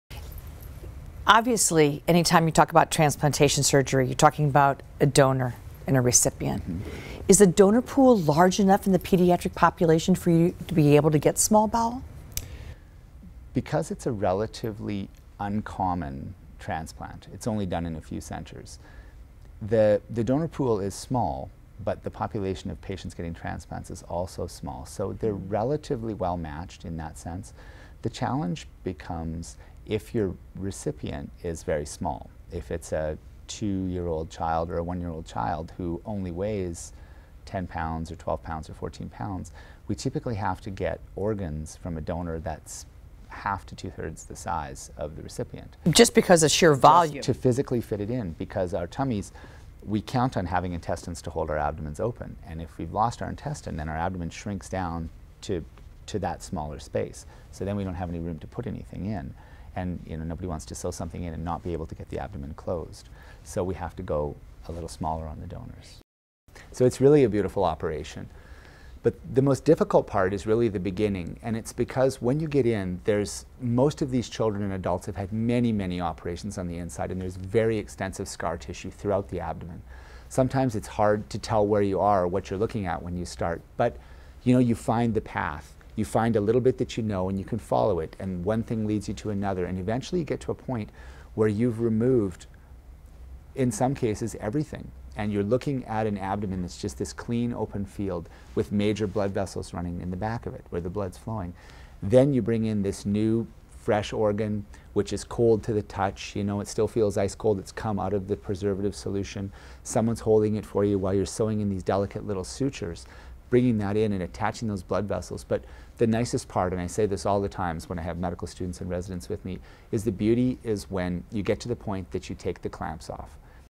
David Mercer, M.D., F.A.C.S., pediatric small bowel transplants, rehabilitation
Examples of media interviews featuring David F. Mercer, M.D., F.A.C.S., of the University of Nebraska Medical Center. Dr. Mercer is an expert in pediatric small bowel transplants and is director of the Intestinal Rehabilitation Program.